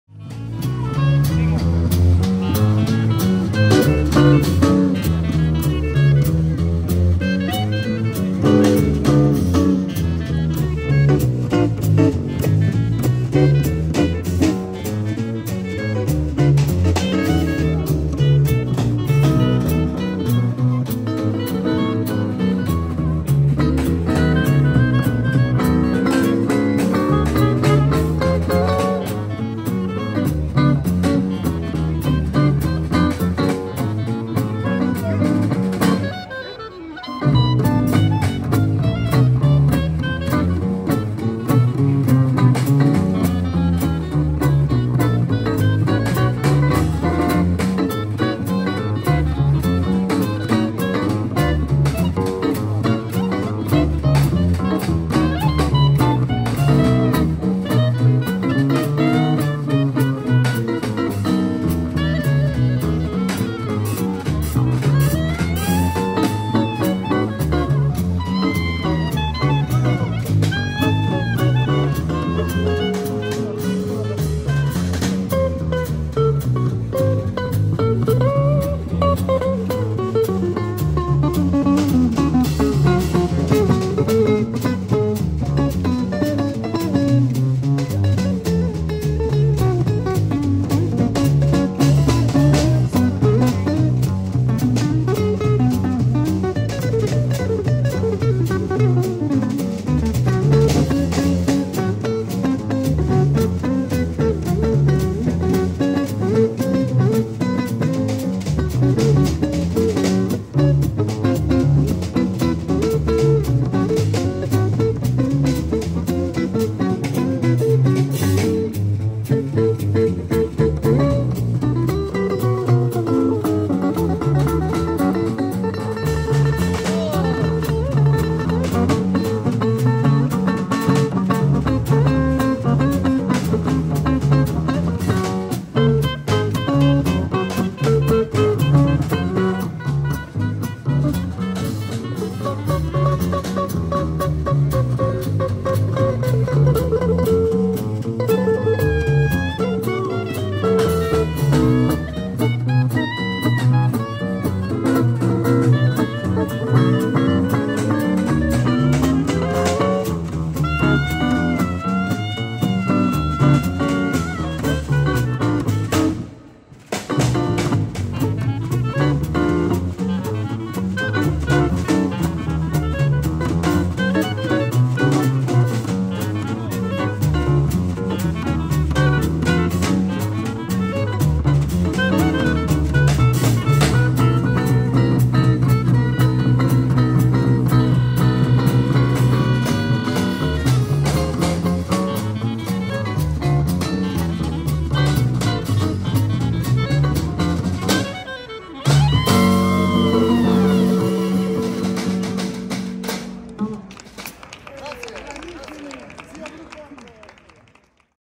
Zima Blue Jazz Quartet live in the street
Zima Blue Jazz Quartet. Busker Jazz in Trieste.

Zima Blue is a jazz musical project borned in 2019, Trieste.
Members:
Tinto Maestri - Clarinet
Simone Di Gennaro - Guitar
Emanuele Goio - Bass
Davide Fradeloni - Drums

Shot on iPhone 13 pro max by Beppe Vergara